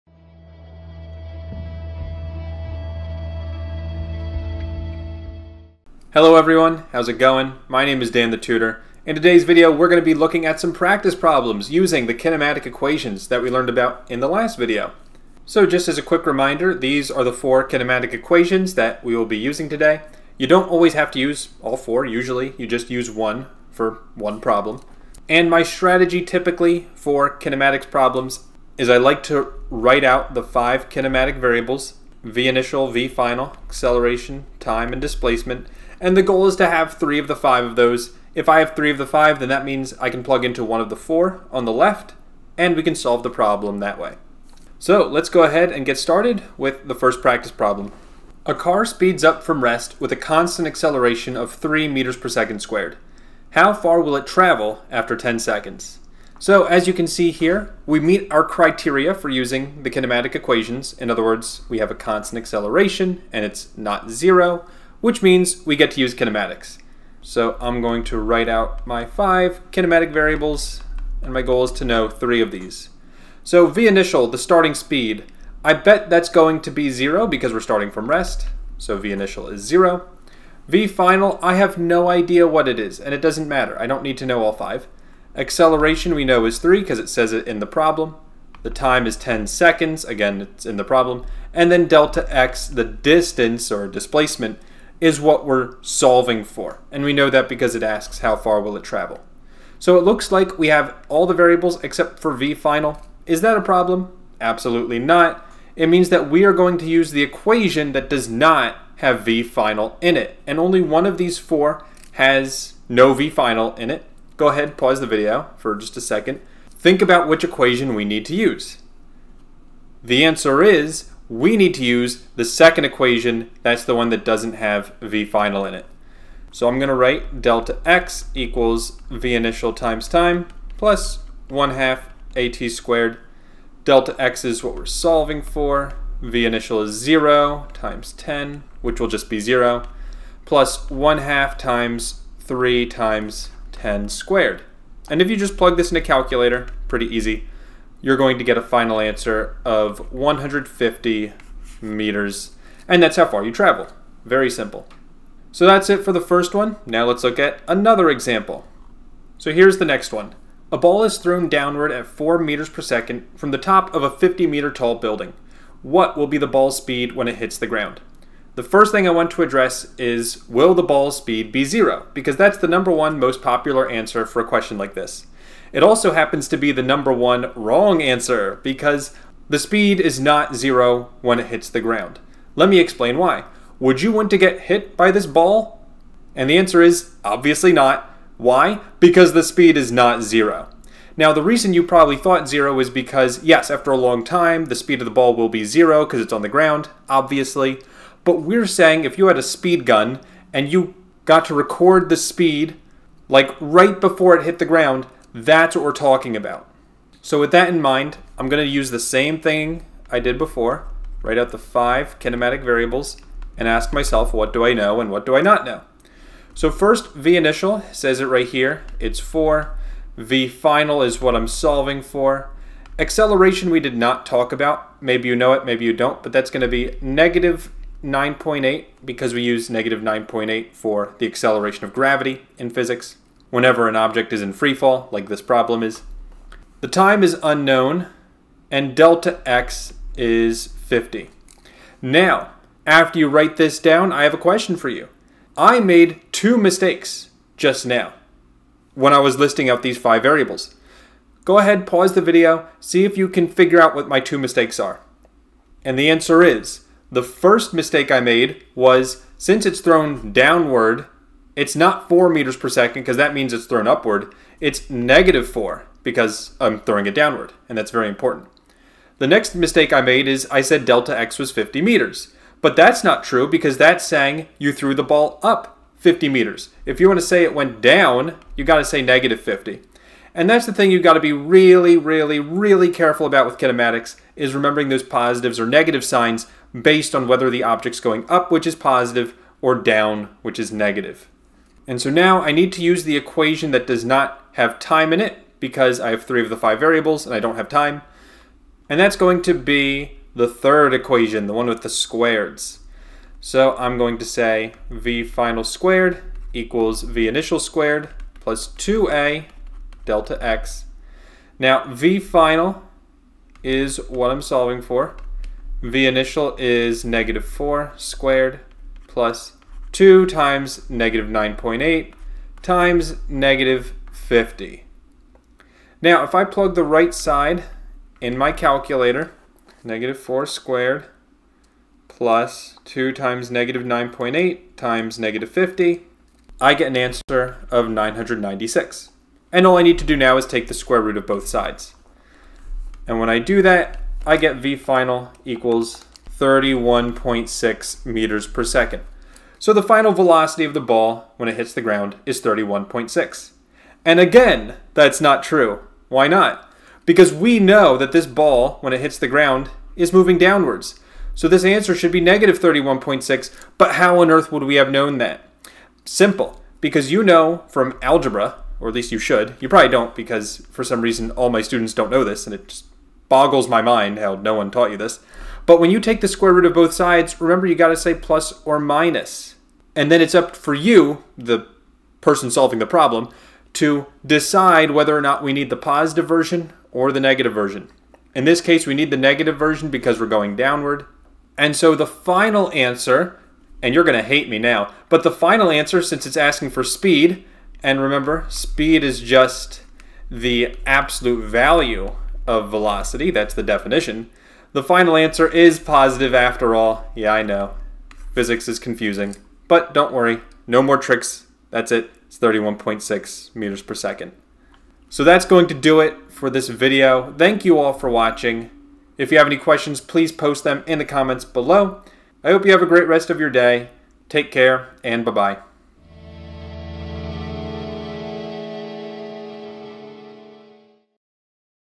Solving Kinematics Problems in Physics (1D Motion)
I explain how to solve physics problems using the kinematic equations. This is also known as 1D motion.